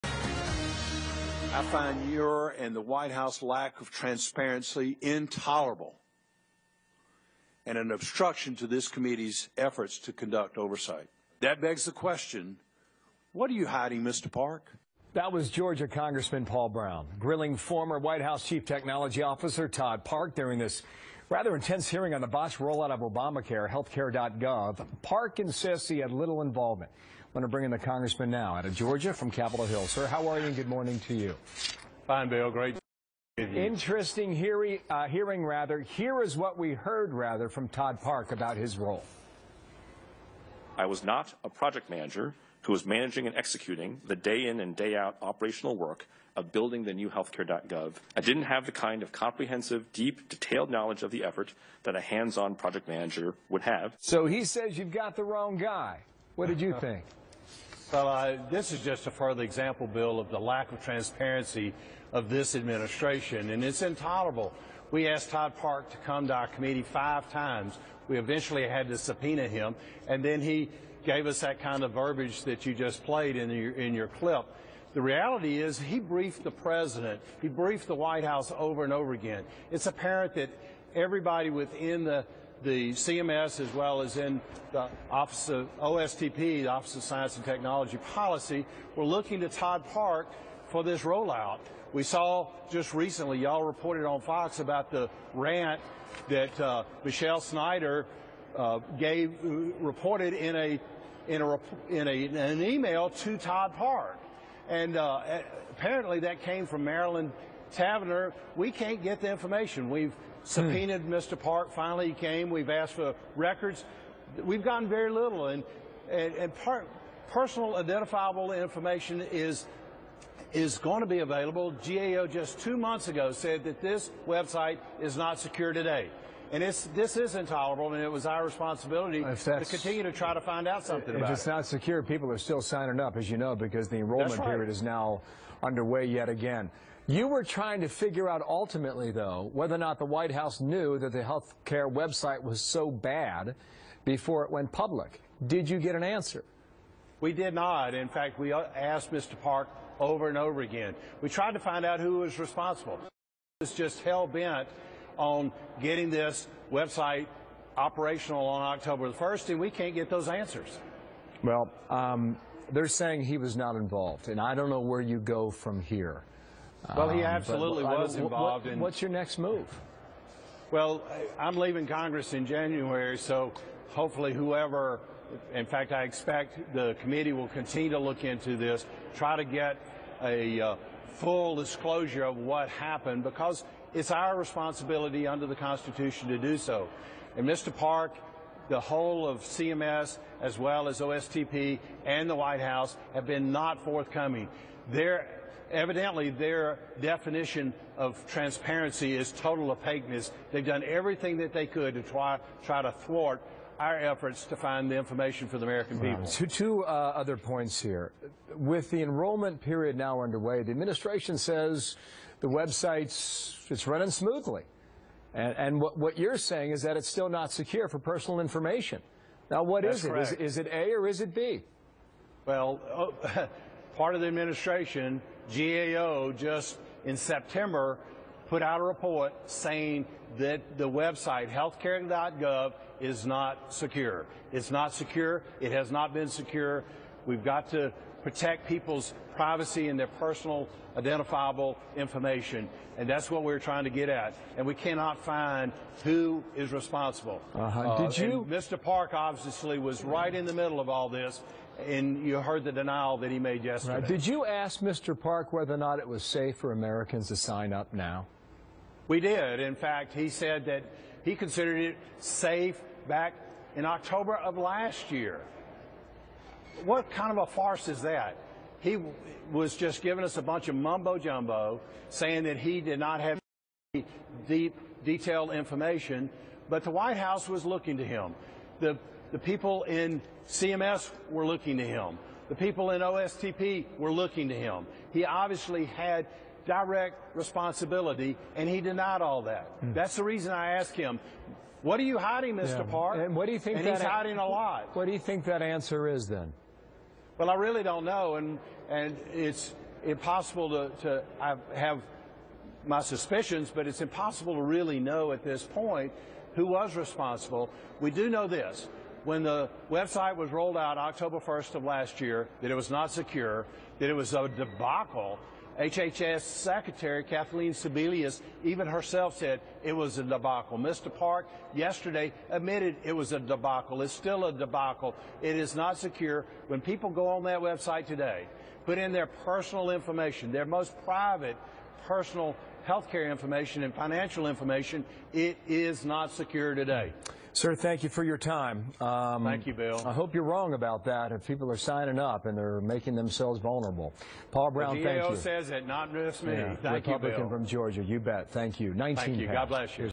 Fox News: Rep Paul Broun on HealthCare.gov security
Oversight Subcommittee Chairman Paul Broun (R-Ga.) on what the President knew about security problems with the Obamacare website.